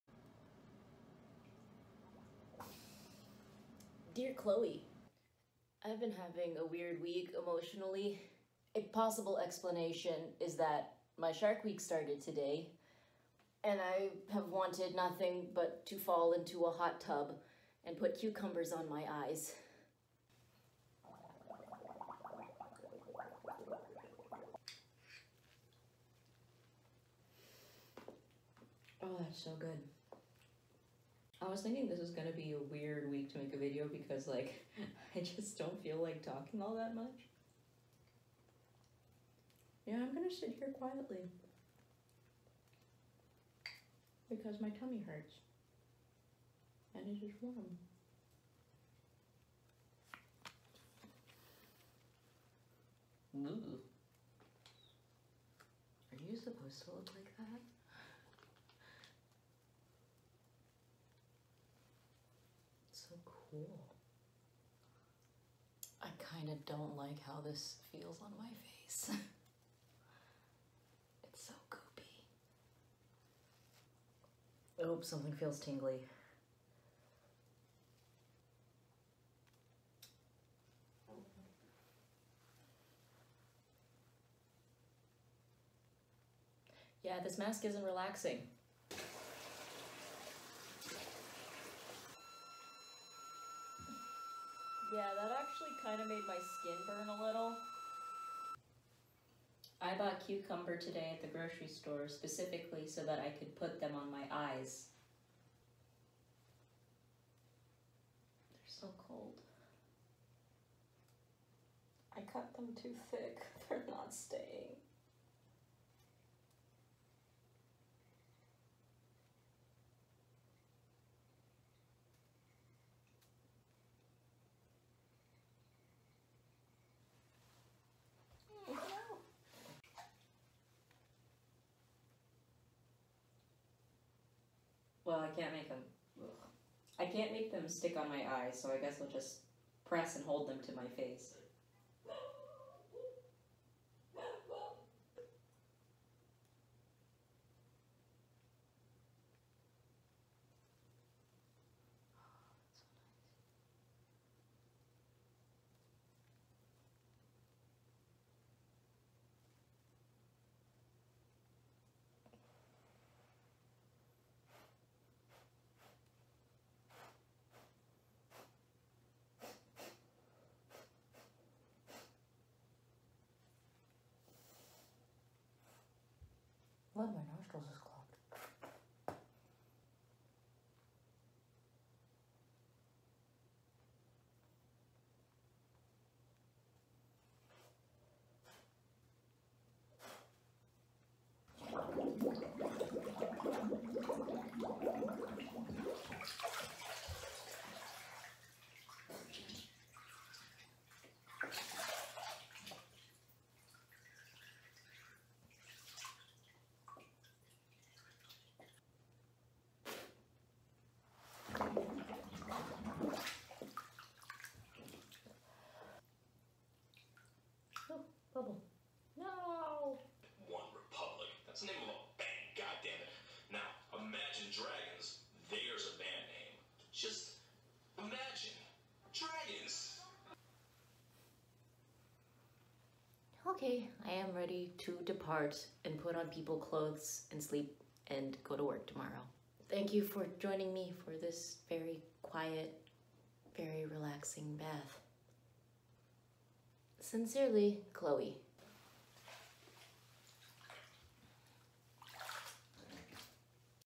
Taking a Relaxing Bath
In which I take a quiet bath.  That's it, that's the video.  :)